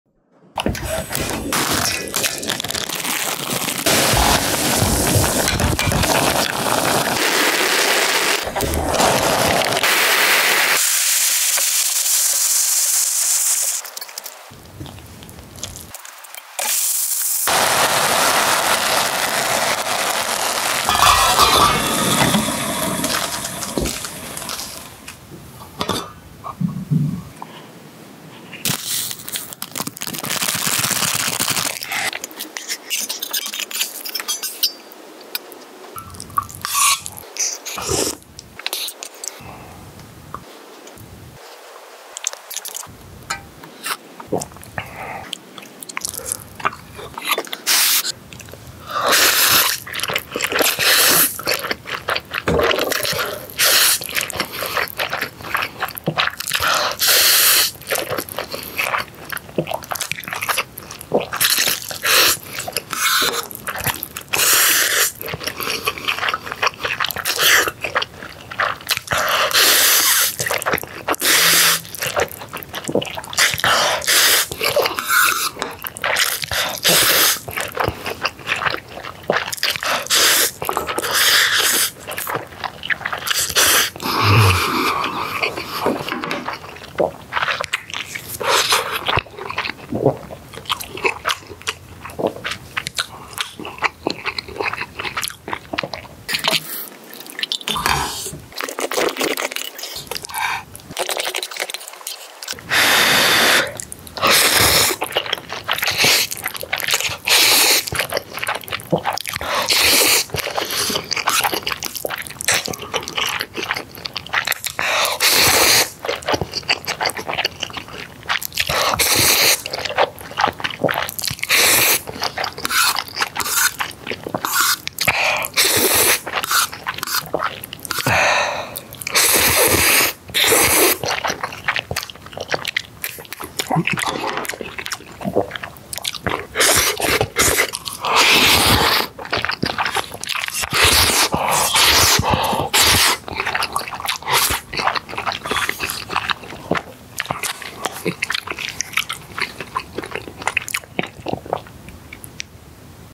ASMR 짜파게티 1봉 불닭볶음면 1봉 1달걀 2치즈 완벽해! JJAPAGHETTI AND BULDAK SPICY NOODLES ARE JUST PERFECT!
짜파게티 1봉 불닭볶음면 1봉 1달걀 2치즈 (1 PACK JJAPAGHETTI, 1 PACK BULDAK SPICY NOODLES, 1 EGG, 2 CHEESE) ASMR eating show